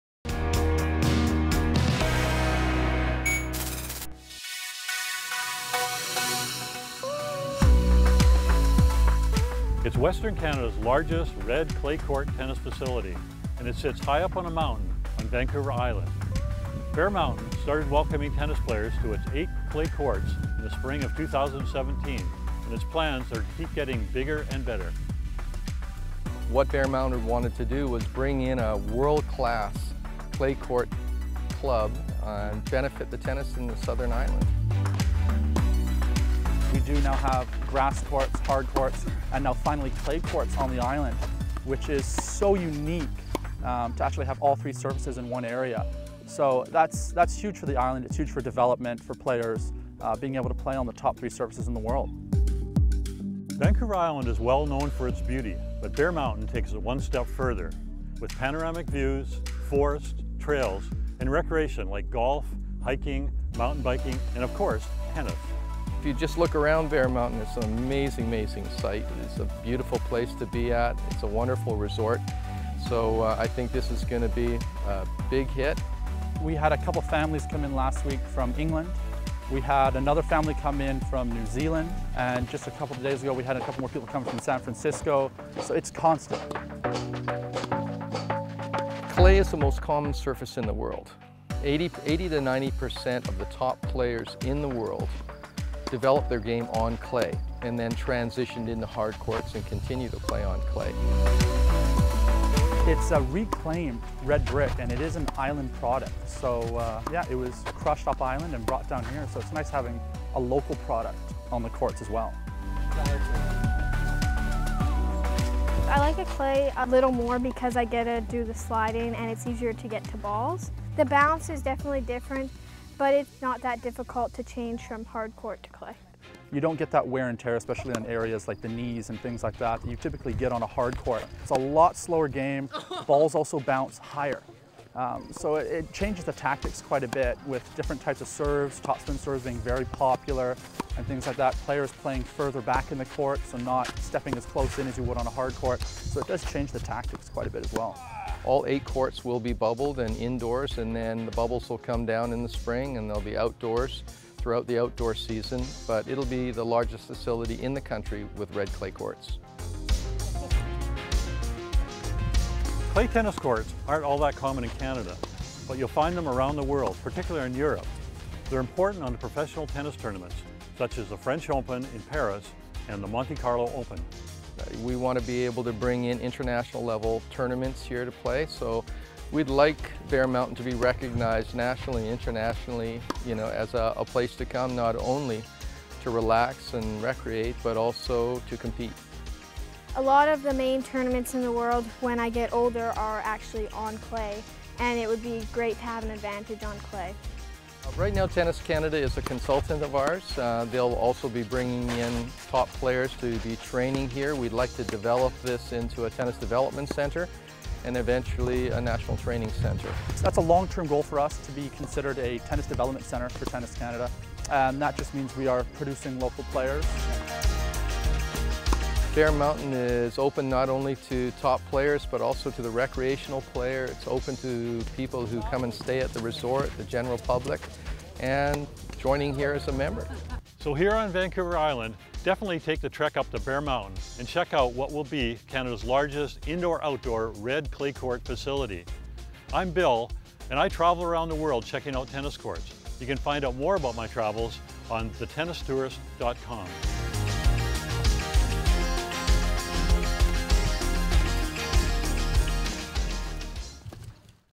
Bear Mountain Tennis
Besides being a beautiful place to visit, Bear Mountain just outside of Victoria, now has 8 clay tennis courts. They hope to attract professional tennis players as well as recreational users and also hold tournaments.